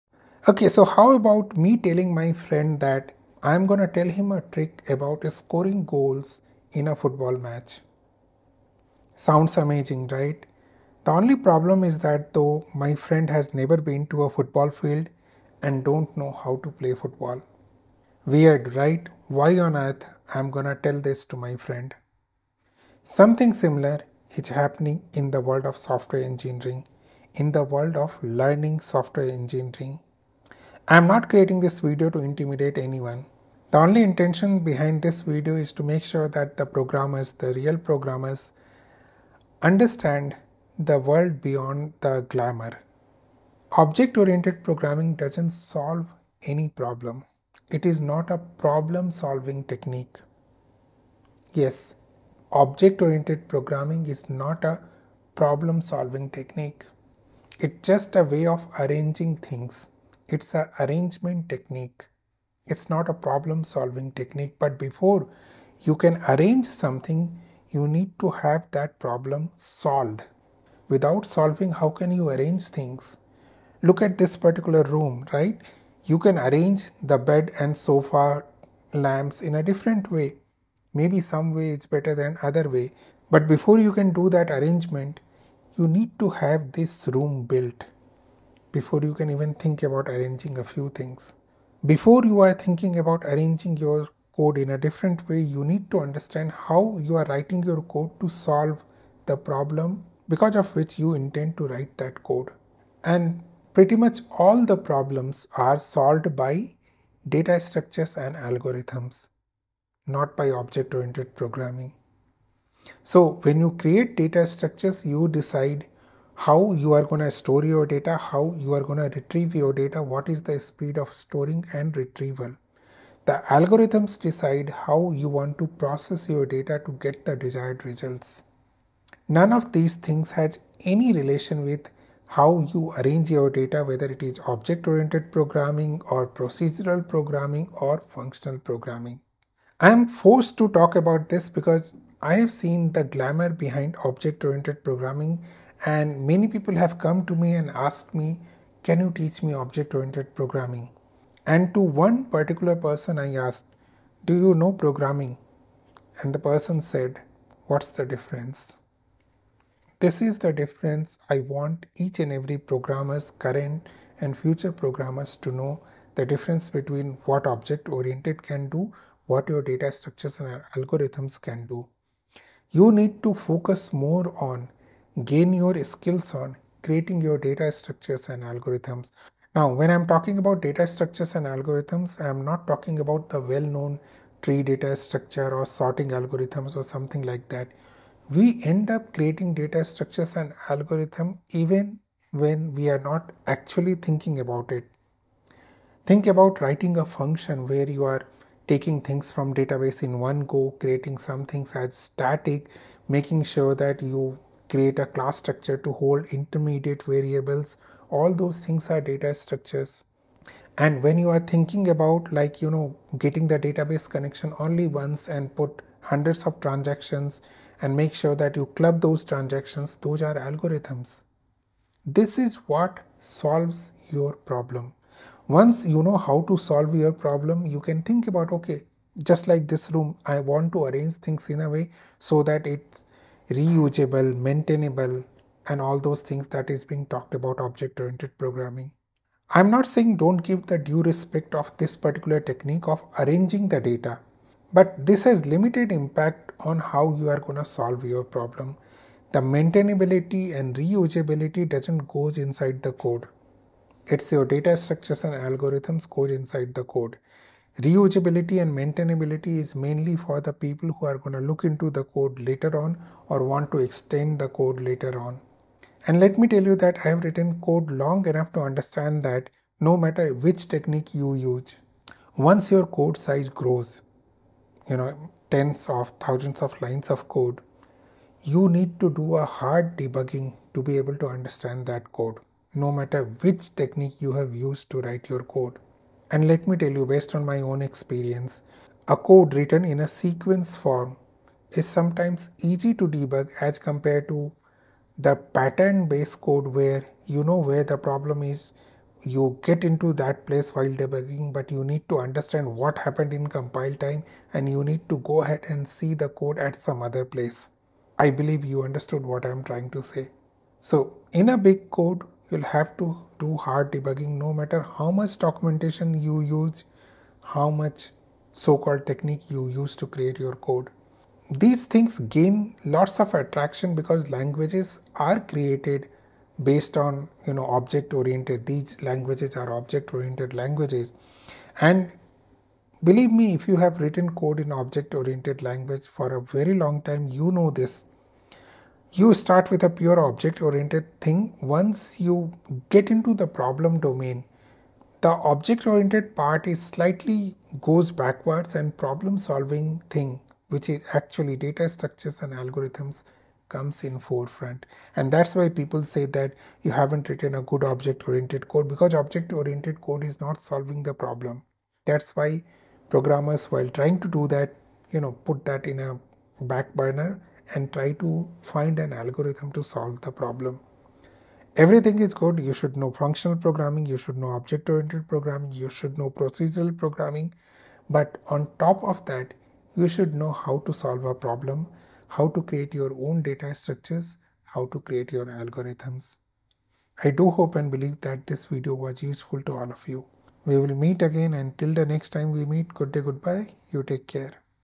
Object Oriented Programming is NOT a Problem Solving Technique
Object oriented programming is an unnecessary distraction to problem during the stage of problem solving because object oriented programming or OOP is not intended to solve a problem but intends to arrange the data and the functions in a different way that what is being done by procedural programming or functional programming.

However, the glamour behind object oriented programming is such that people today equates object oriented programming with problem solving which its not.

The actual problem in the software is solved by data structures and algorithms, its the data structures which decided how the data will be stored and retrieved and its the algorithms which will decide how we process the data to achieve the desired results.

We need to have our functionality with us before we can think about the arrangement of data and yes object oriented programming is not exactly programming but data structures and algorithms are

I hope and believe that this video was helpful to all of you and you'll like this video